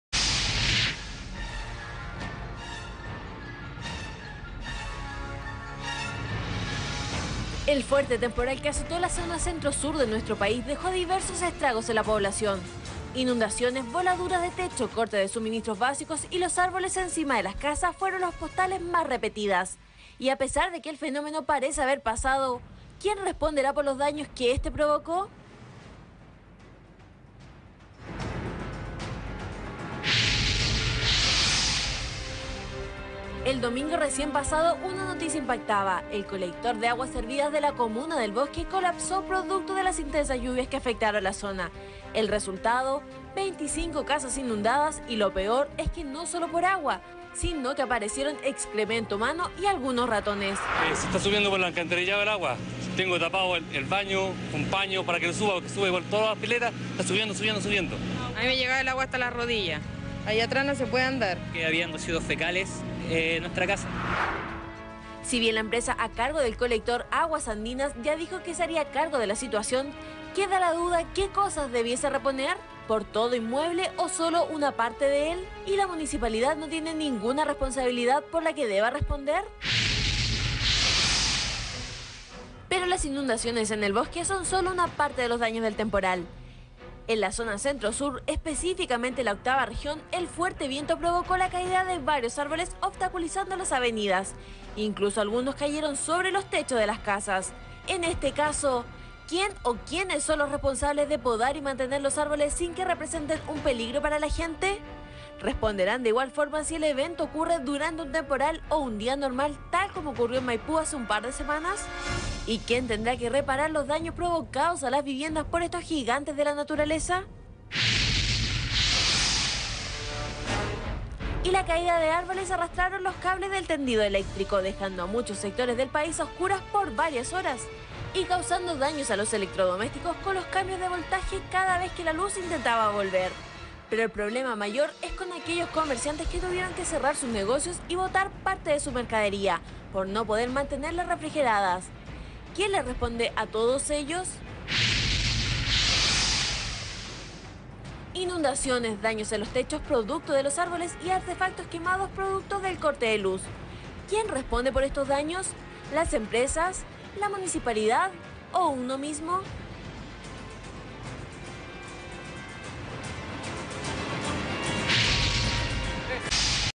¿Qué hacer si se cae un poste? ¿Quien responde?
Quien asume los costos